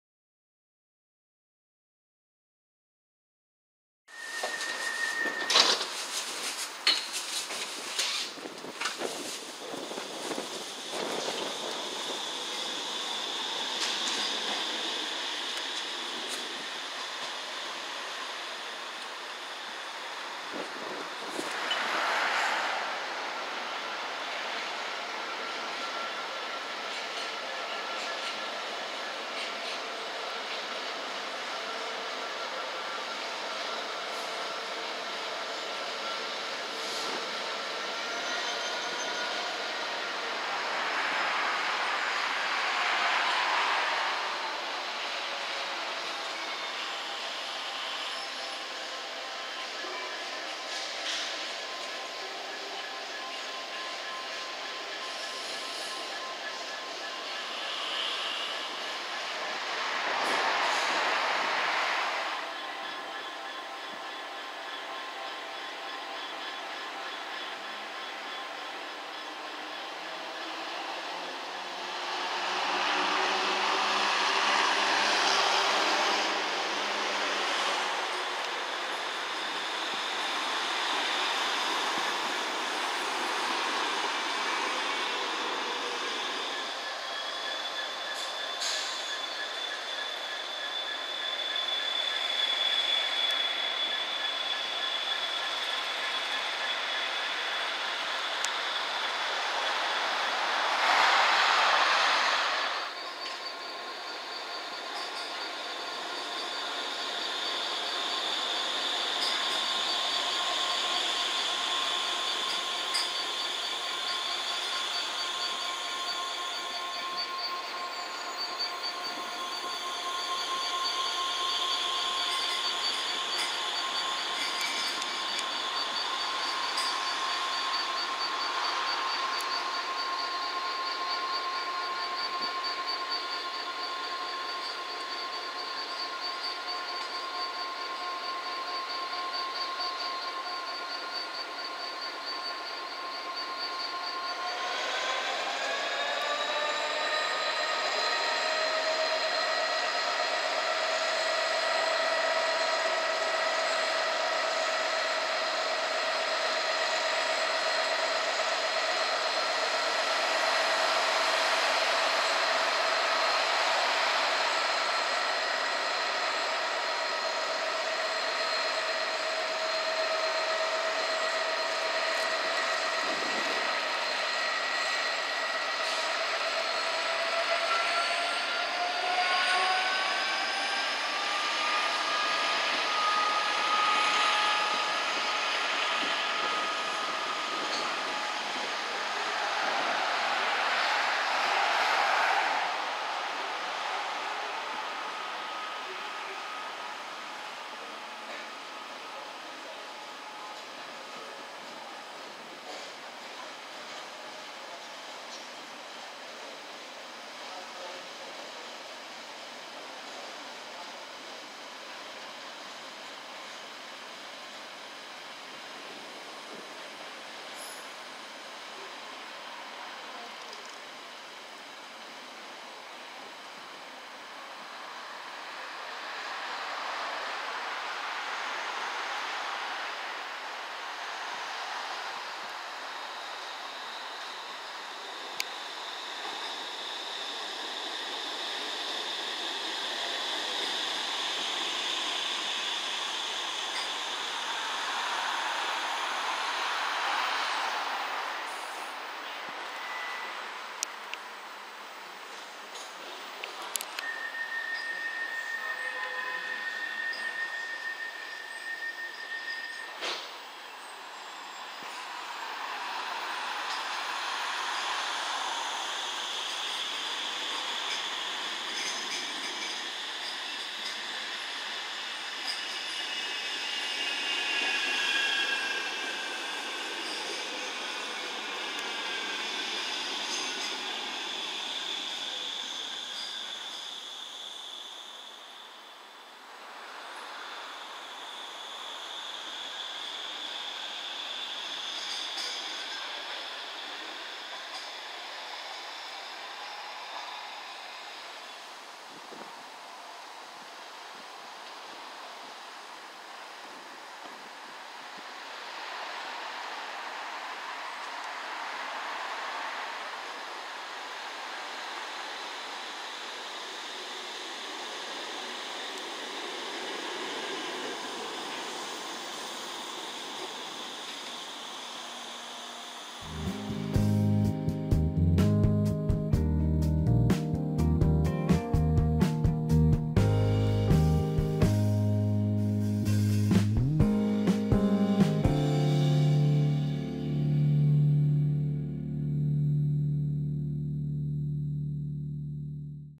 Trams at Central Park
Includes:
Class 66
Class 150
M5000

TOCs Include:
Northern
Metrolink